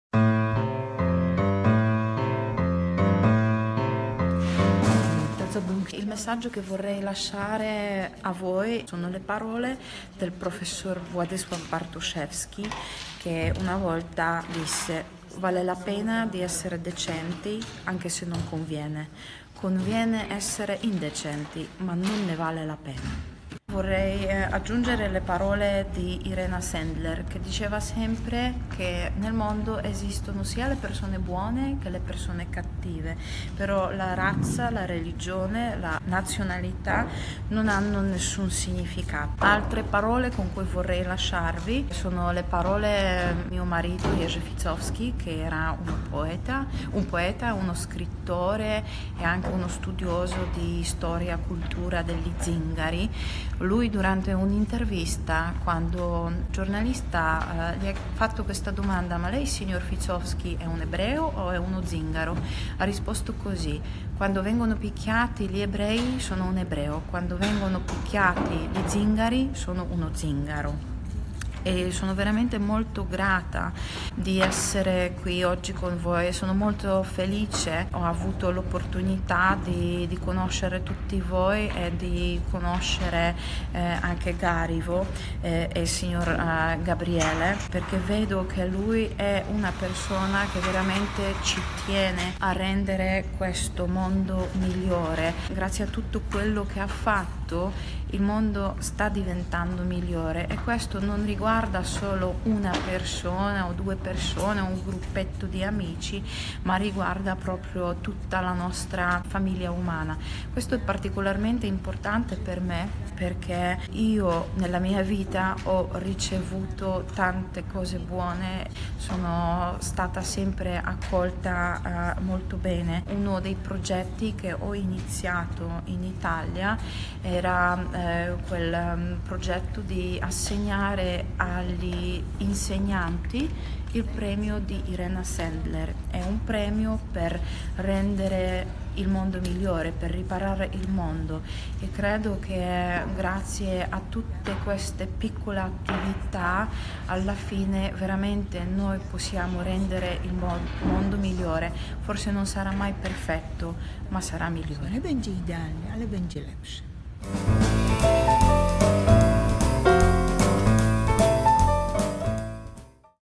Messaggio di Elżbieta Ficowska
Abbiamo avuto con noi, in occasione del Giorno della Memoria 2020 al teatro PIME, Elżbieta Ficowska, la bambina più piccola del ghetto di Varsavia salvata da Irena Sendler.